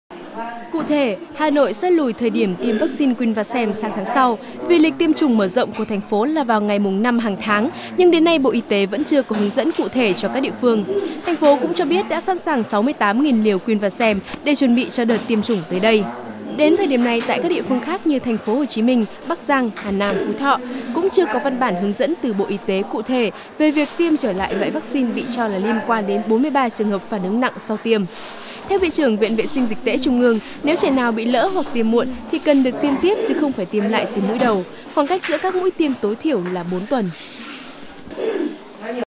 VTC14_Hà Nội: Tháng 11 mới tiêm lại vắc xin Quinvaxem_03.10.2013
Hà Nội: Tháng 11 mới tiêm lại vắc xin Quinvaxem:
Theo quyết định của Bộ Y tế, văcxin Quinvaxem sẽ được sử dụng trở lại trong chương trình tiêm chủng mở rộng trên toàn quốc từ tháng 10/2013, tuy nhiên, tại Hà Nội, sớm nhất là từ đầu tháng 11/2013, loại văcxin gây nhiều tranh cãi này mới được tiêm trở lại.